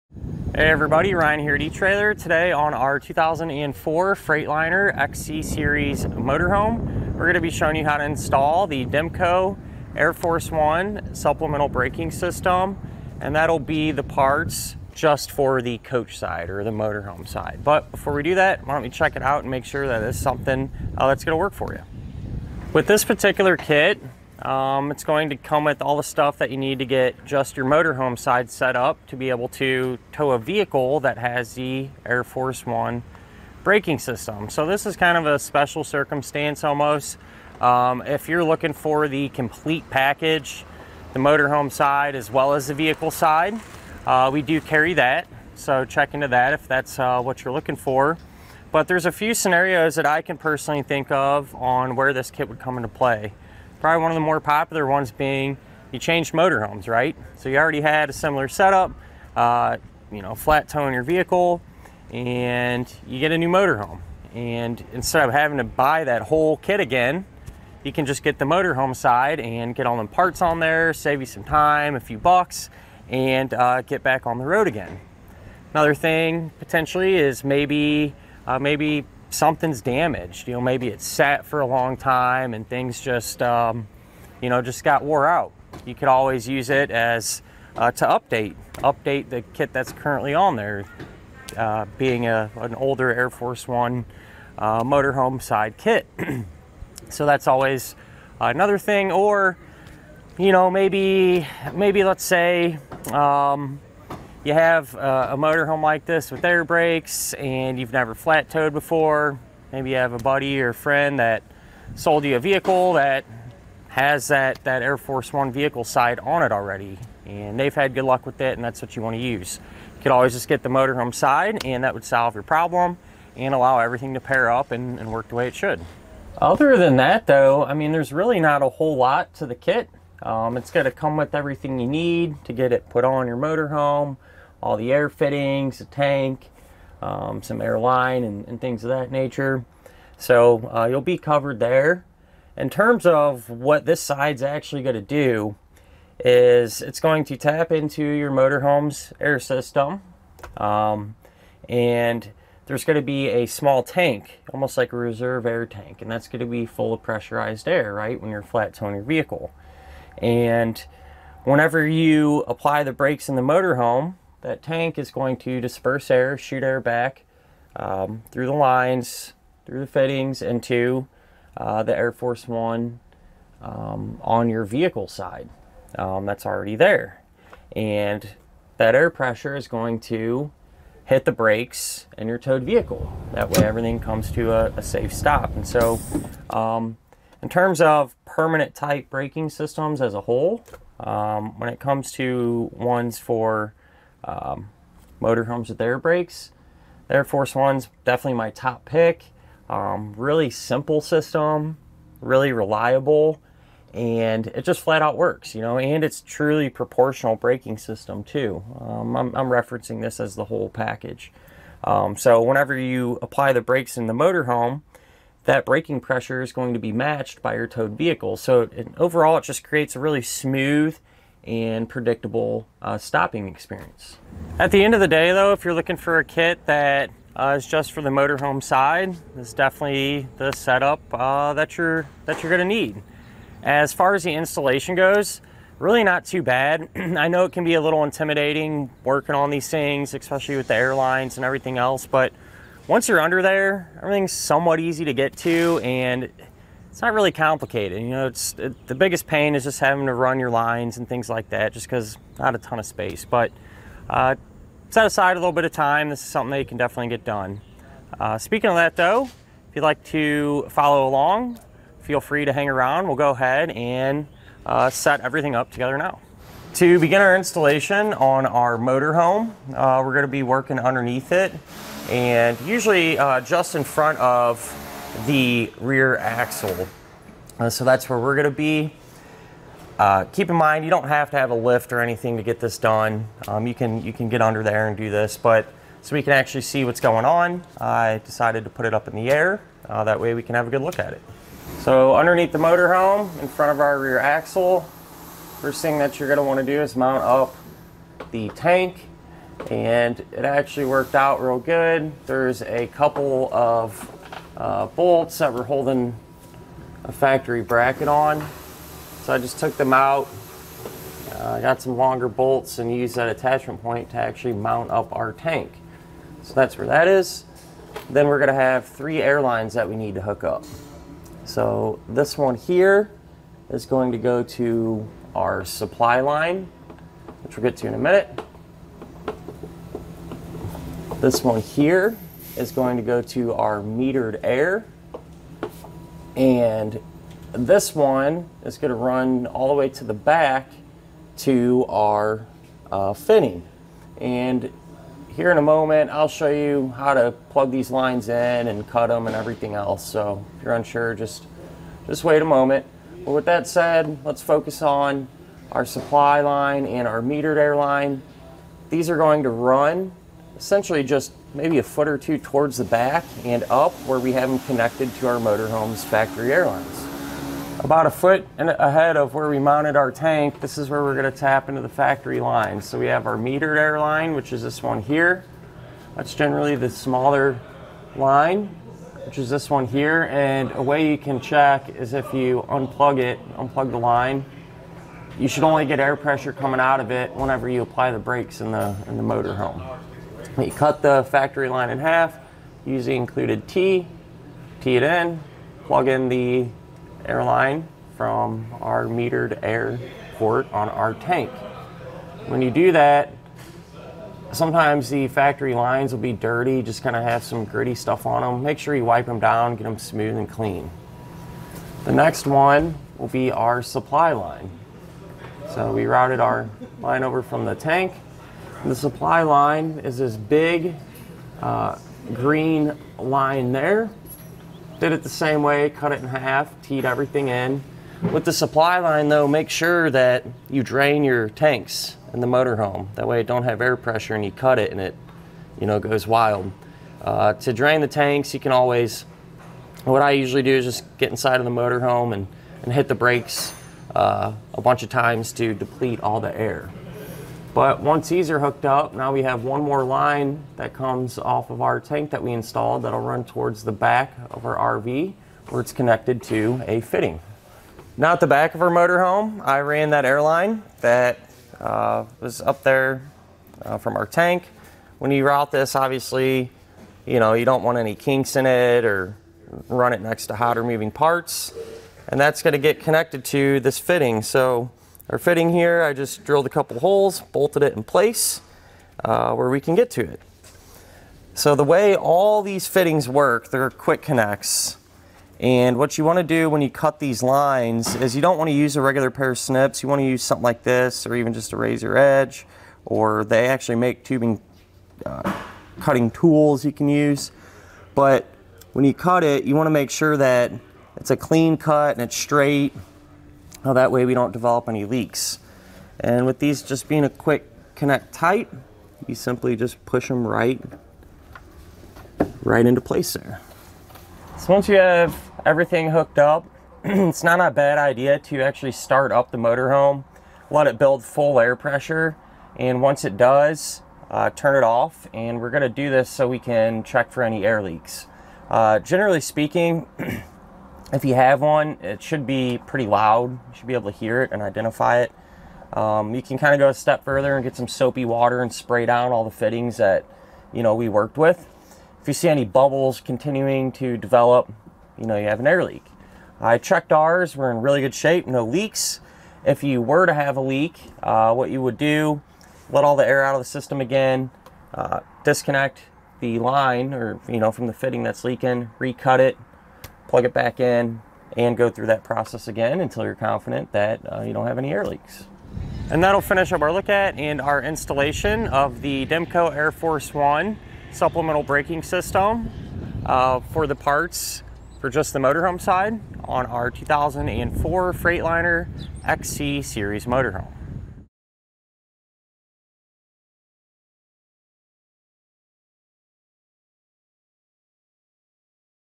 etrailer | Demco SBS Air Force One Coach Air Kit Installation - 2004 Freightliner XC-Series
Click for more info and reviews of this Demco Flat Tow Brake System:

Hey everybody, Ryan here at etrailer. Today on our 2004 Freightliner XC Series motor home we're gonna be showing you how to install the DemCo Air Force One supplemental braking system. And that'll be the parts just for the coach side, or the motor home side. But before we do that, why don't we check it out and make sure that it's something that's gonna work for you. With this particular kit, it's going to come with all the stuff that you need to get just your motor home side set up to be able to tow a vehicle that has the Air Force One breaking system. So this is kind of a special circumstance almost, if you're looking for the complete package, the motor home side as well as the vehicle side, we do carry that.

So check into that if that's what you're looking for. But there's a few scenarios that I can personally think of on where this kit would come into play. Probably one of the more popular ones being you change motor homes, right So you already had a similar setup, flat towing your vehicle, and you get a new motor home. And instead of having to buy that whole kit again, you can just get the motor home side and get all the parts on there, save you some time, a few bucks, and get back on the road again. Another thing potentially, is maybe something's damaged.

You know, maybe it's sat for a long time and things just got wore out. You could always use it to update the kit that's currently on there, being a an older Air Force One motor home side kit. So that's always another thing. Or maybe let's say you have a motor home like this with air brakes and you've never flat towed before, maybe you have a buddy or friend that sold you a vehicle that has that Air Force One vehicle side on it already and they've had good luck with it and that's what you want to use. You could always just get the motor home side and that would solve your problem and allow everything to pair up and work the way it should.

Other than that though, I mean there's really not a whole lot to the kit. It's gonna come with everything you need to get it put on your motor home, all the air fittings, the tank, some airline, and things of that nature. So you'll be covered there. In terms of what this side's actually gonna do is it's going to tap into your motor home's air system, and there's gonna be a small tank, almost like a reserve air tank, and that's gonna be full of pressurized air when you're flat towing your vehicle. And whenever you apply the brakes in the motor home, that tank is going to disperse air, shoot air back through the lines, through the fittings and to the Air Force One on your vehicle side that's already there.

And that air pressure is going to hit the brakes in your towed vehicle, that way everything comes to a safe stop. And so in terms of permanent tight braking systems as a whole, when it comes to ones for motor homes with air brakes, Air Force Ones, definitely my top pick. Really simple system, really reliable, and it just flat out works, and it's truly proportional breaking system too. I'm referencing this as the whole package. So whenever you apply the brakes in the motor home, that breaking pressure is going to be matched by your towed vehicle. So overall, it just creates a really smooth and predictable stopping experience. At the end of the day, though, if you're looking for a kit that is just for the motor home side, it's definitely the setup that you're gonna need. As far as the installation goes, really not too bad. I know it can be a little intimidating working on these things, especially with the airlines and everything else, but once you're under there, everything's somewhat easy to get to and it's not really complicated. The biggest pain is just having to run your lines and things like that, just 'cause not a ton of space. But set aside a little bit of time, this is something they can definitely get done. Speaking of that though, if you'd like to follow along, feel free to hang around, we'll go ahead and set everything up together now. To begin our installation on our motor home, we're gonna be working underneath it and usually just in front of the rear axle, so that's where we're gonna be. Keep in mind, you don't have to have a lift or anything to get this done you can get under there and do this. But, so we can actually see what's going on, I decided to put it up in the air that